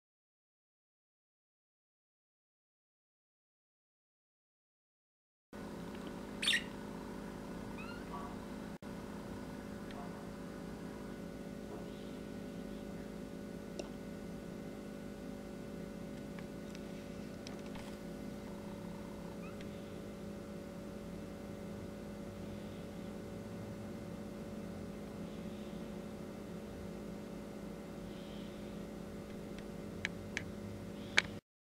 budgie learning to talk 'BUDGIE WHISPERER'
'BUDGIE WHISPERER'

even if I move my mouth,, my budgie will respond to me.. its a good sign.. that she is learning to talk or even have any interest..